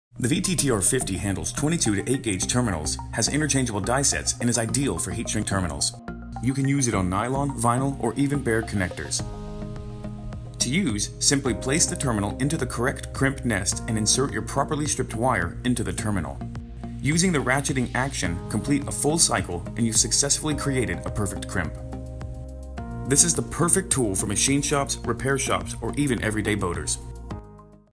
Pacer's VTTR-50 Wire Crimping Tool
Using the proper tool guarantees the correct crimp each and every time. Don't risk damaging your wire or insulation by using the wrong tools. This crimper handles 22-8 AWG terminals and is ideal for high shrink terminals. Additionally, it can crimp nylon, vinyl, and even bare connectors.